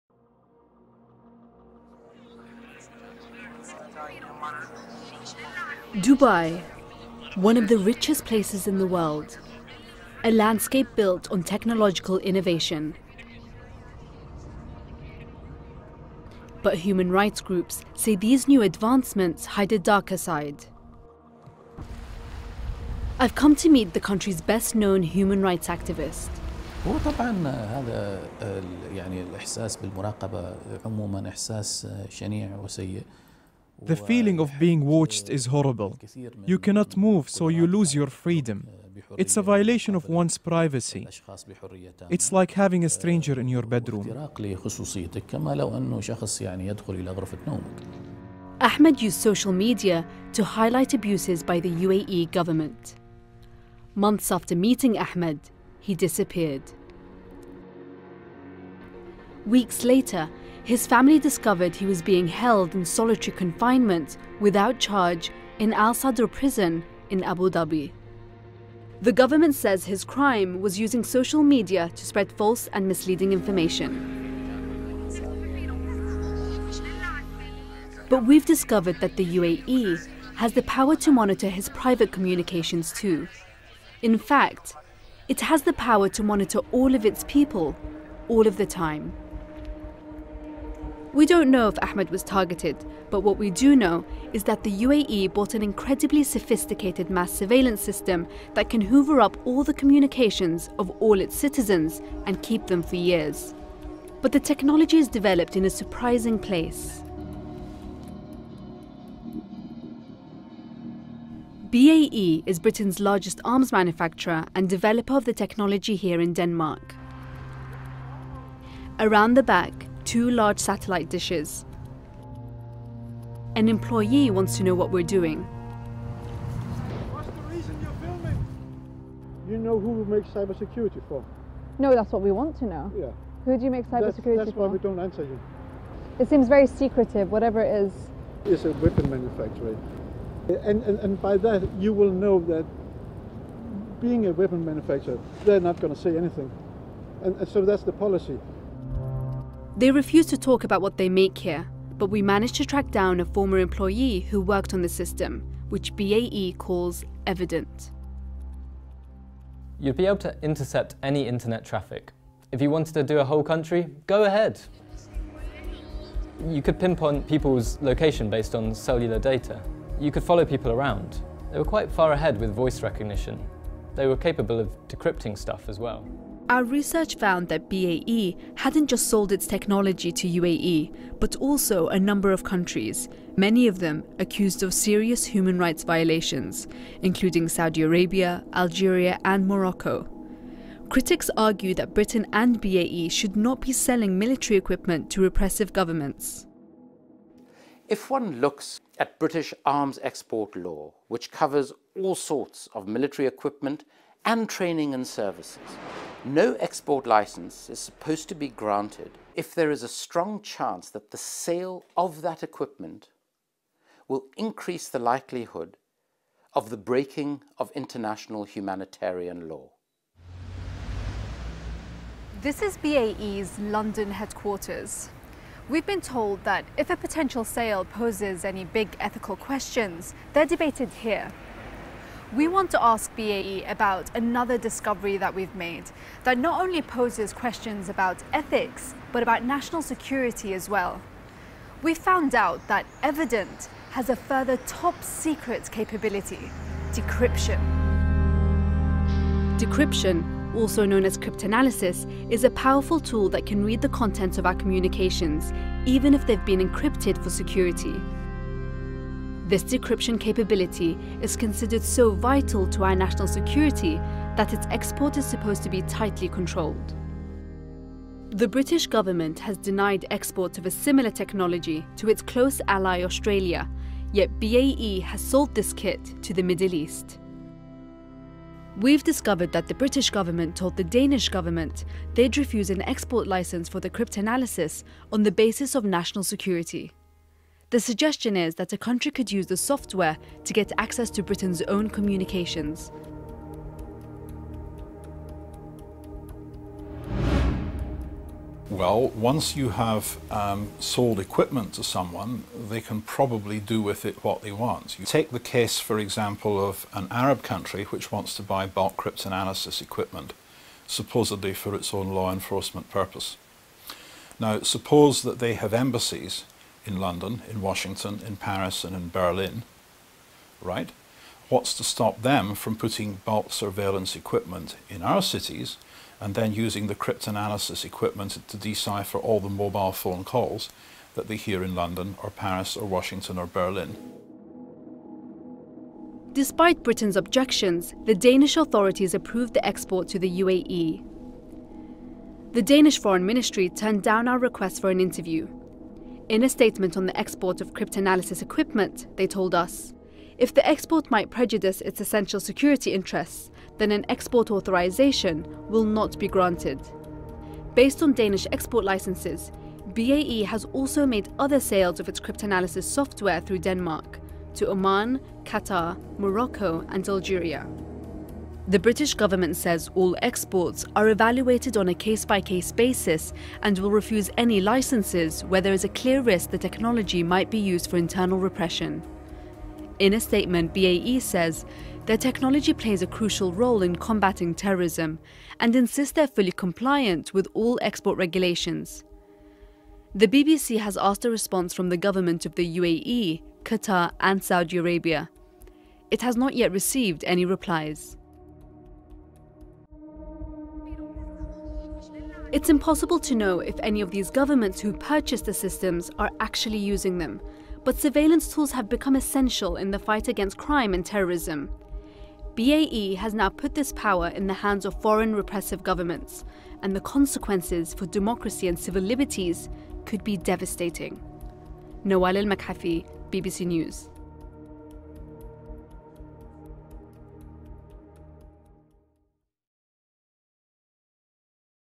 How BAE sold cyber-surveillance tools to Arab states - BBC News
A year-long investigation by BBC Arabic and a Danish newspaper has uncovered evidence that the UK defence giant BAE Systems has made large-scale sales across the Middle East of sophisticated surveillance technology, including to many repressive governments. These sales have also included decryption software which could be used against the UK and its allies.

Much of this technology, which is covered by the same international laws as military equipment, has come from a British company.  It's an important tool in the fight against terrorism but as Nawal Al Maghafi reports it may have been used by repressive governments to silence dissent and human rights campaigners...